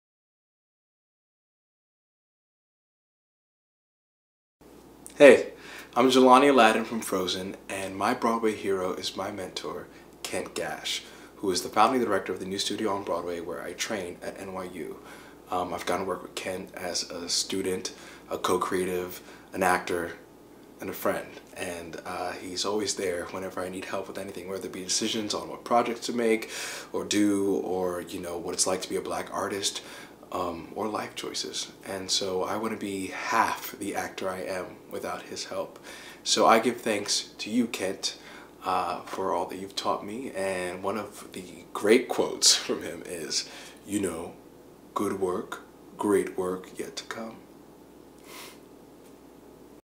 My Broadway Hero - FROZEN's Jelani Alladin
This Thanksgiving we asked cast members from Broadway shows in NYC and on National Tour to give thanks to their Broadway Heroes, people who mentored or inspired them in their Broadway careers.